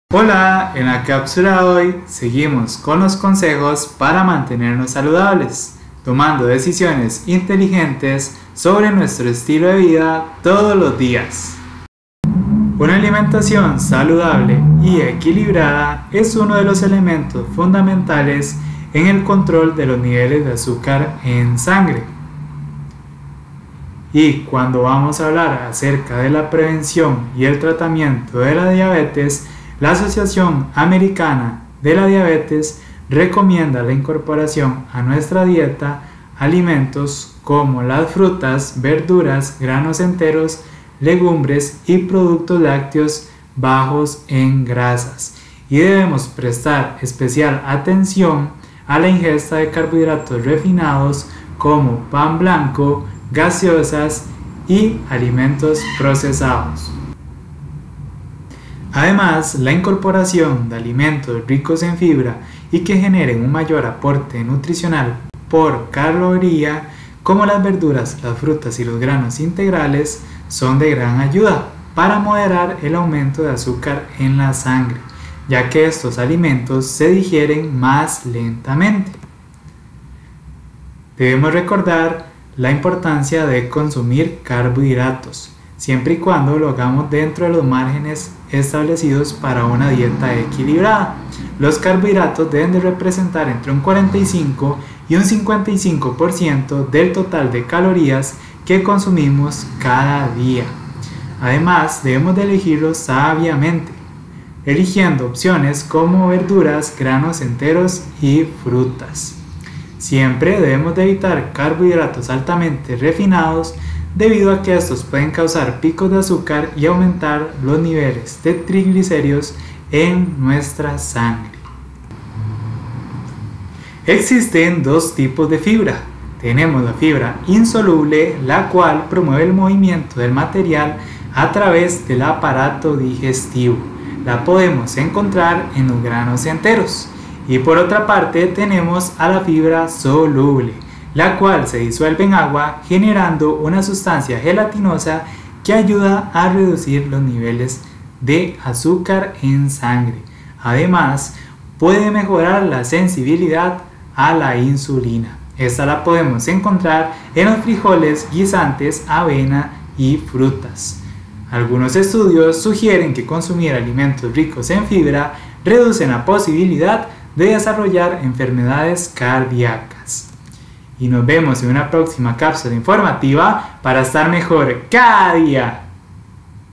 Cápsula de Salud #62 - Una alimentación saludable para niveles óptimos de azúcar en sangre
En la cápsula de hoy, vamos a conocer como una alimentación saludable y equilibrada es uno de los elementos fundamentales en el control de los niveles de azúcar en sangre.